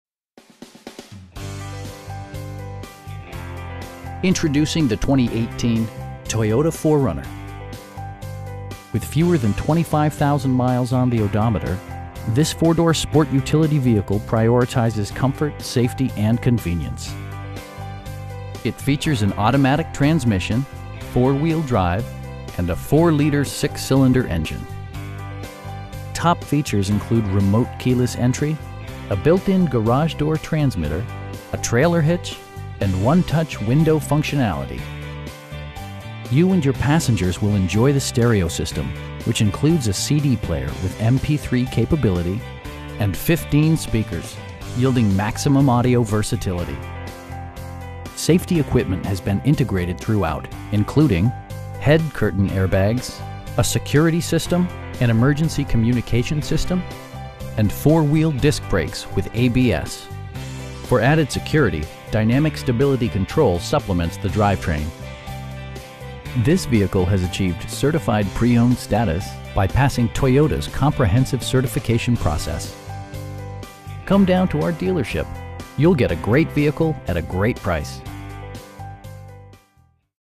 2018 Toyota 4Runner Limited in Greenville, SC 29607
Introducing the 2018 Toyota 4Runner. With fewer than 25,000 miles on the odometer, this 4 door sport utility vehicle prioritizes comfort, safety and convenience. It features an automatic transmission, 4-wheel drive, and a 4 liter 6 cylinder engine. Top features include remote keyless entry, a built-in garage door transmitter, an automatic dimming rear-view mirror, power moon roof, skid plates, an overhead console, a trailer hitch, and 1-touch window functionality. Features such as automatic climate control and leather upholstery prove that economical transportation does not need to be sparsely equipped. You and your passengers will enjoy the stereo system, which includes a CD player with MP3 capability, steering wheel mounted audio controls, and 15 speakers, providing excellent sound throughout the cabin. Safety equipment has been integrated throughout, including: dual front impact airbags, head curtain airbags, traction control, brake assist, anti-whiplash front head restraints, a security system, an emergency communication system, and 4 wheel disc brakes with ABS. For added security, dynamic Stability Control supplements the drivetrain. This vehicle has achieved Certified Pre-Owned status, by passing Toyota's comprehensive certification process. Come down to our dealership, you'll get a great vehicle at a great price, with the experience and dedication of our whole team behind you.